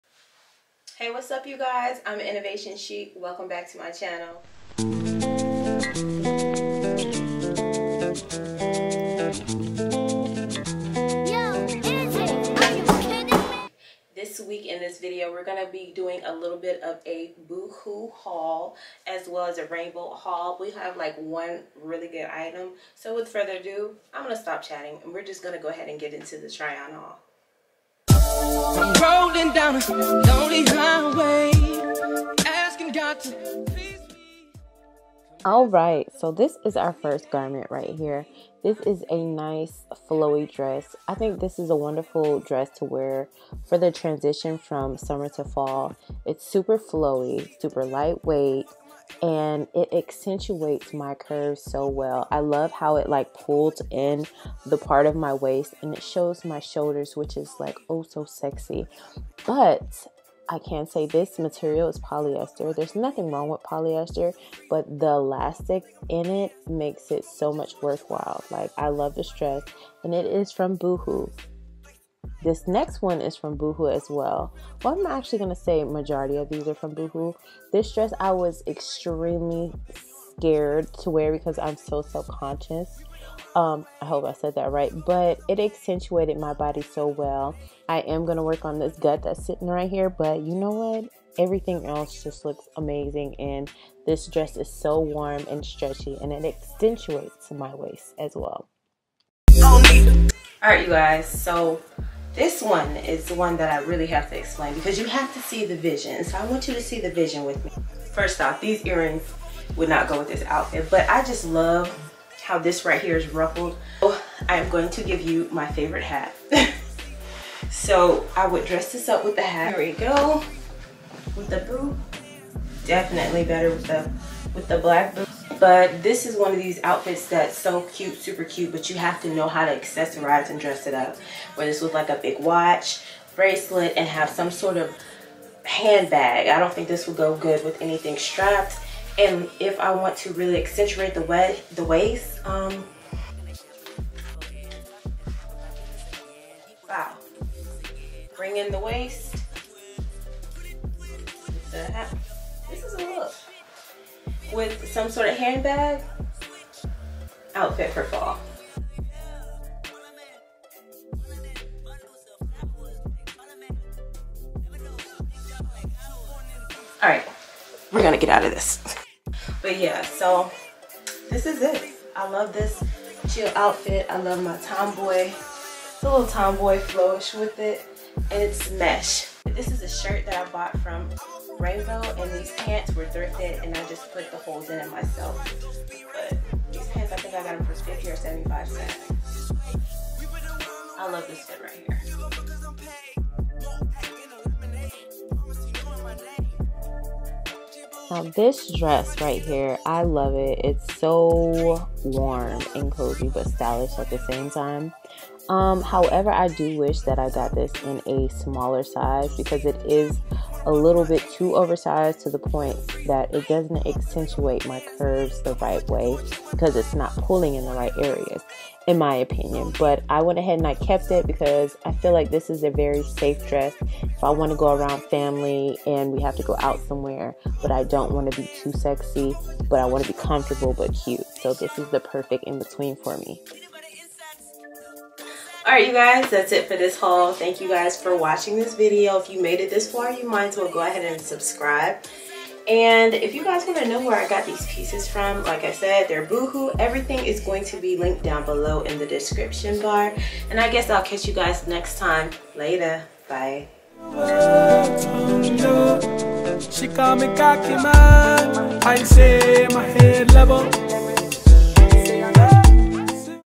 Plus Size Boohoo Haul
This week we’re  doing a boohoo plus size clothing haul . Now in this haul I threw in a thrifted item as well as a Rainbow  item. Can you figure out which one before watching  the video??

Dresses

Green mesh croptop
Rainbow Clearance rack

My lipstain

Camera: Sony ZV-1